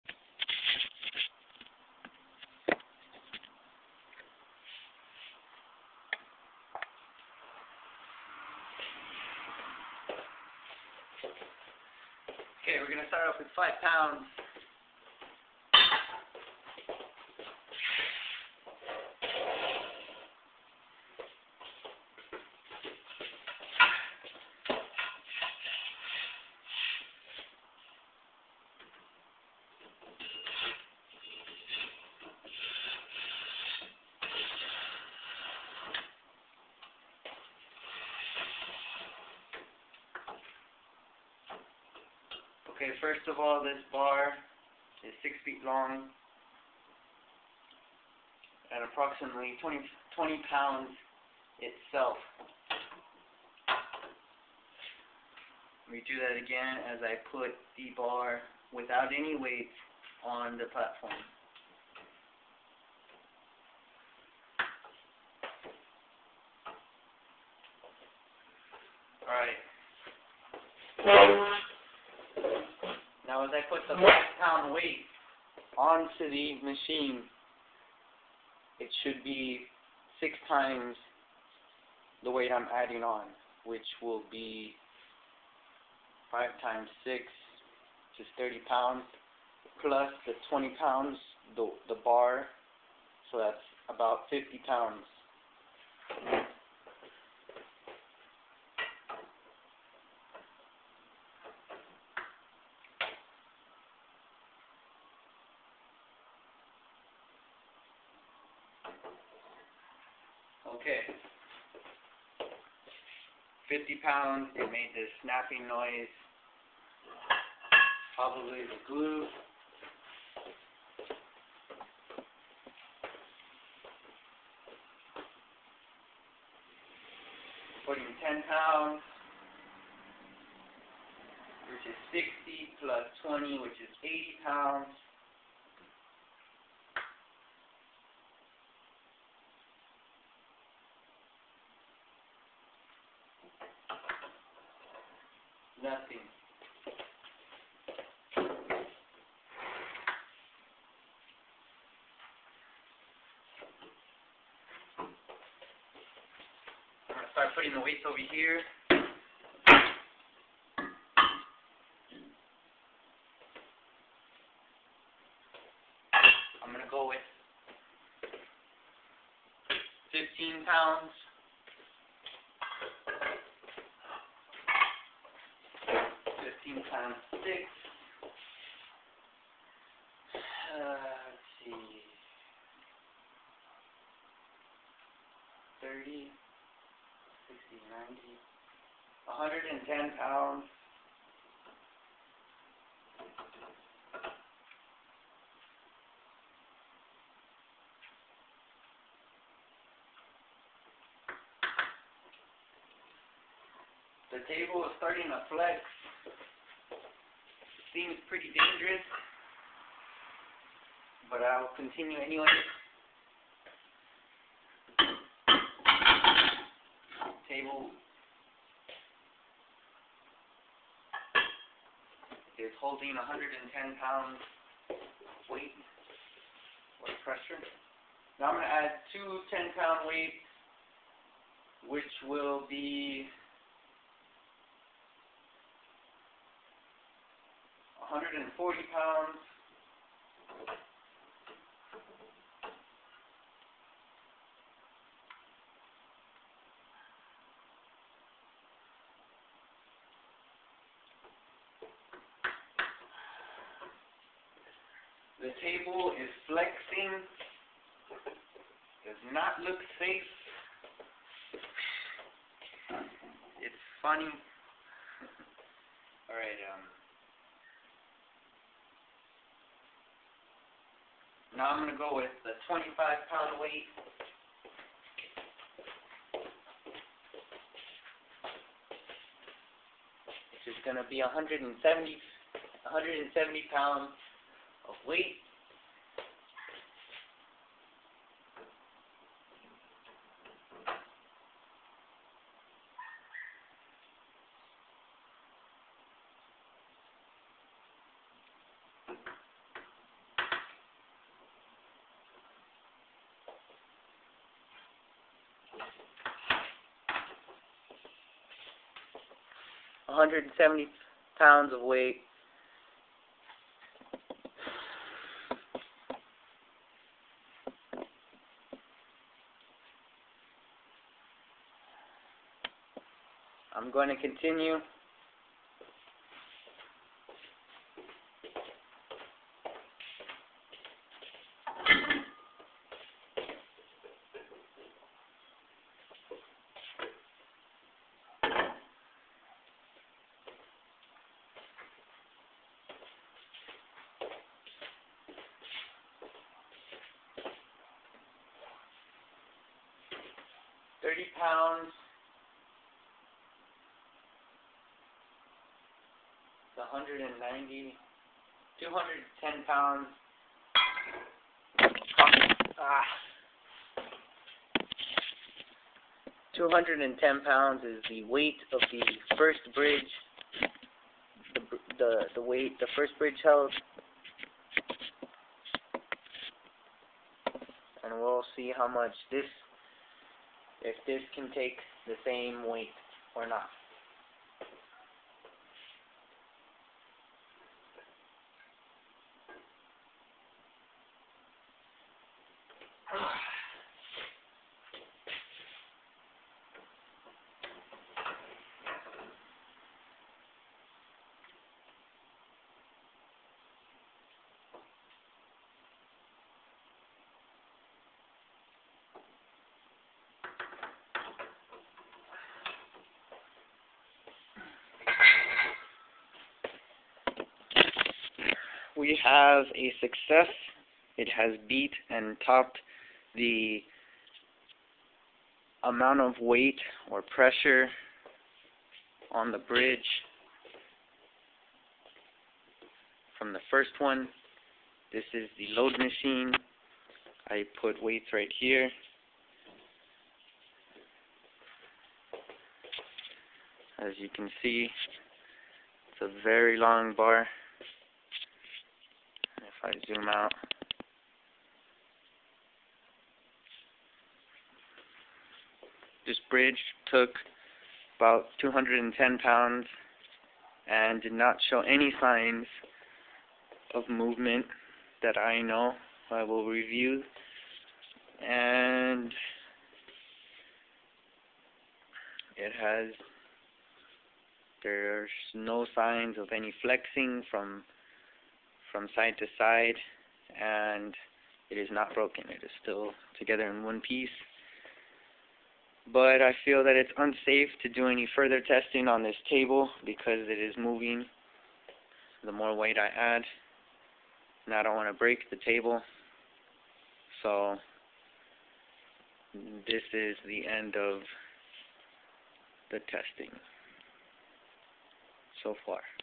2010 Popsicle stick bridge(home test)
I'm involved in a competition with my fellow classmen to represent Bassett High School in the 2010 popsicle stick bridge contest. The final day is on Febuary 19,2010 & i'm just showing a little test video of our second model of the bridge. This contest is in the LA area, so if you live nearby the chances are i'll see u there!